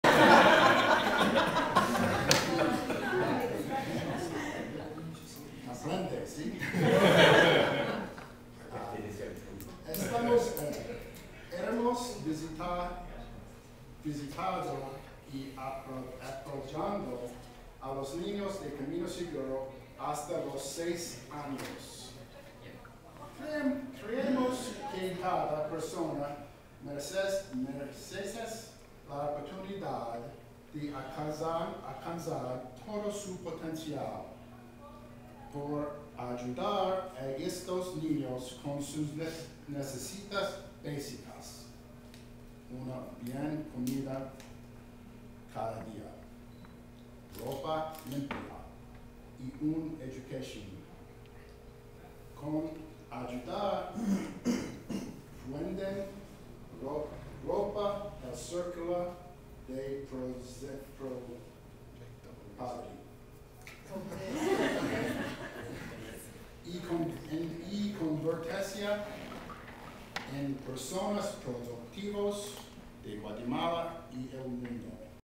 Club Rotario Antigua
Patzún, presentación proyecto de agua en Club Rotario de Antigua